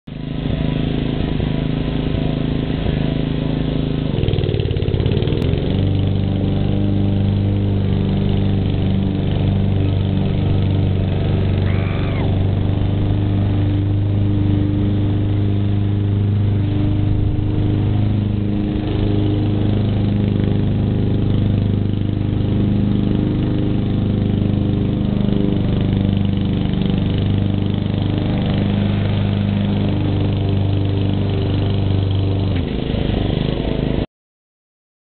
how to cut a lawn the fun way
if u have a self propelled lawnmower then try this idea.